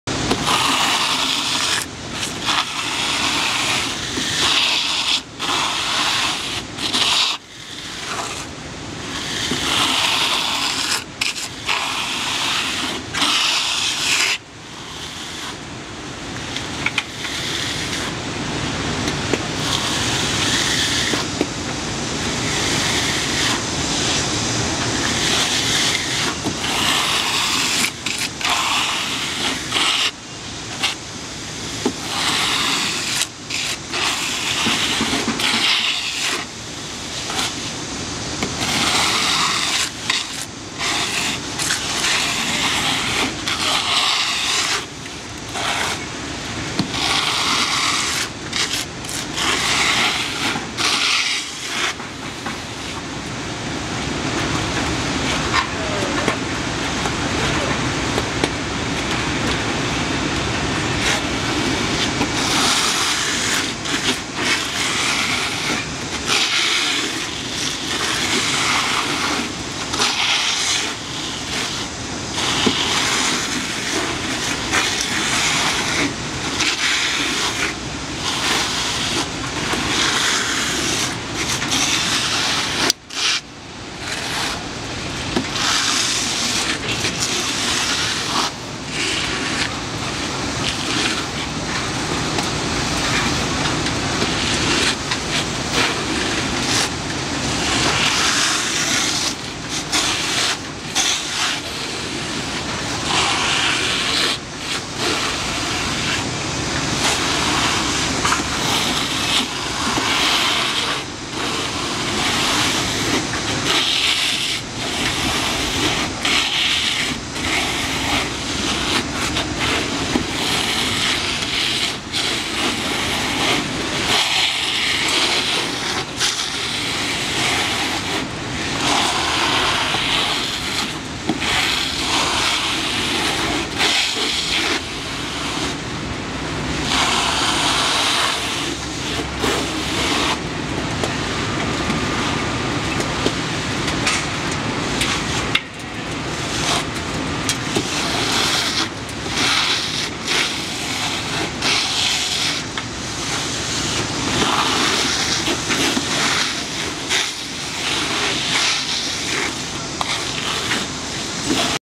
10층 고층 빌딩 병원 리모델링 외단열 시공 28-701 - 스타코 마감, PF보드 단열재 (외벽 드라이비트, 준불연 페놀폼)
building remodeling stucco finish on PF board in Korea